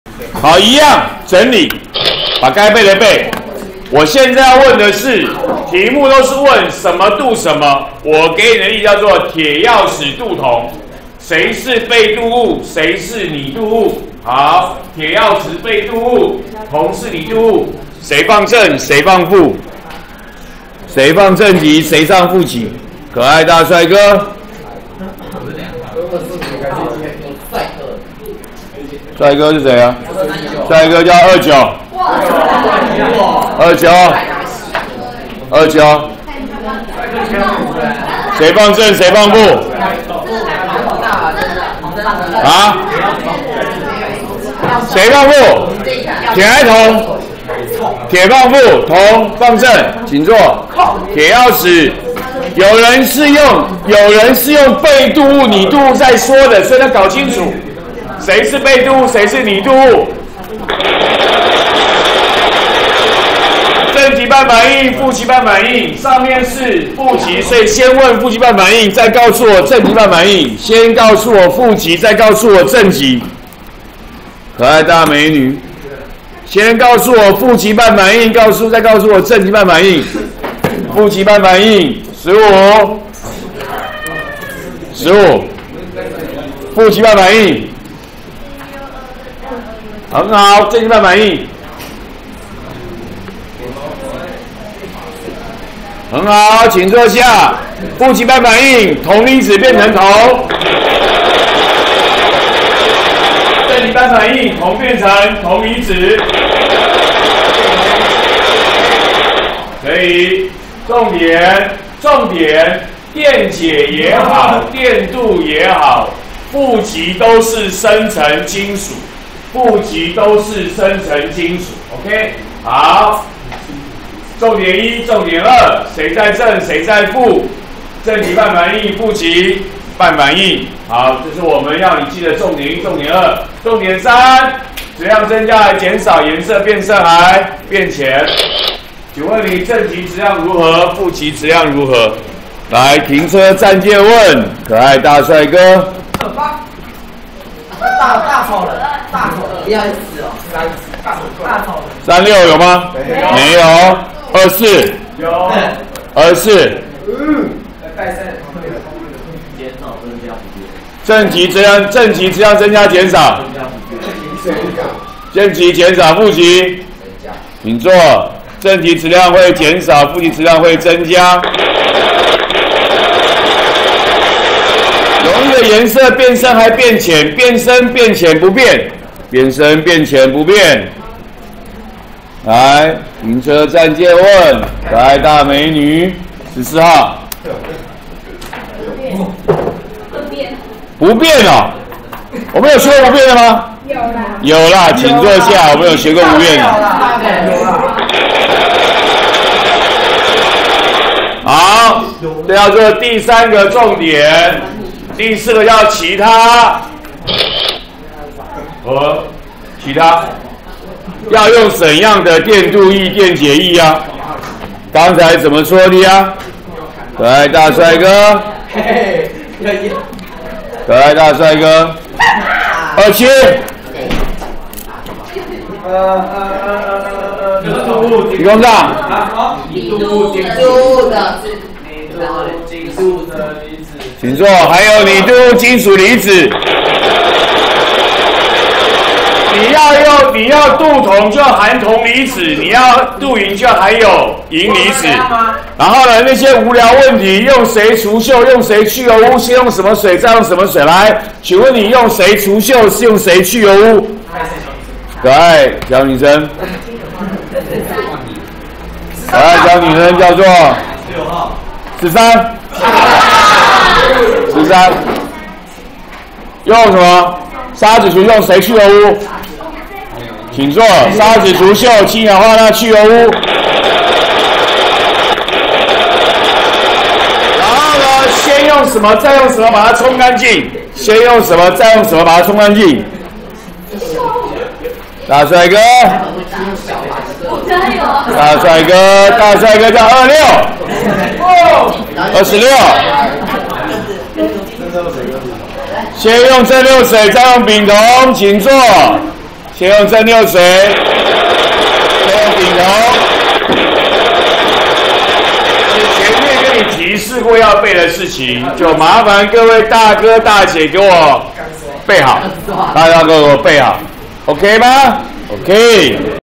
104學年國三理化＿電化學25：電鍍整理
整理並詢問同學關於電鍍應理解、記憶事項。